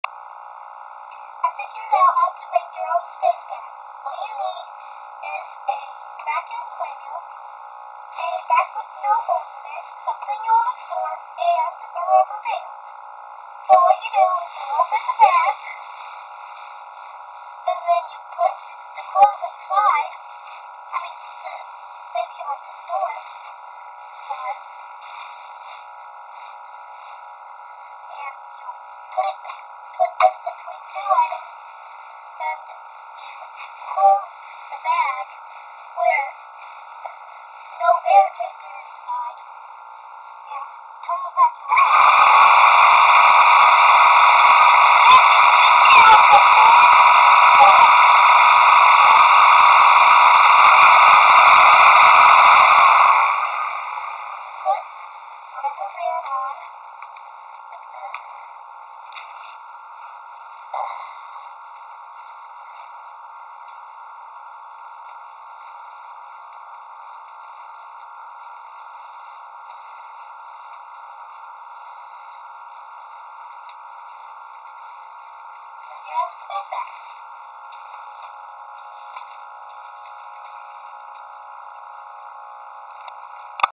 how to make your own space bags
William makes home made space bags